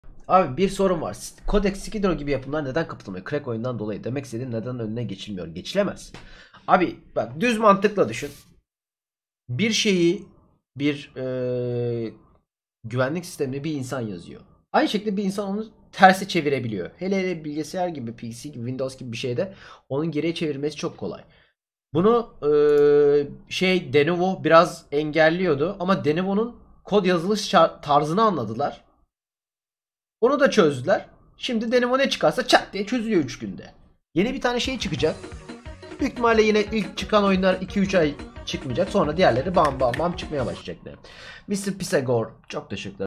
Videoyun-Neden Oyunların Cracklenmesi Önlenemiyor
Videoyun neden oyunların cracklenmesinin önlenemediğini anlatıyor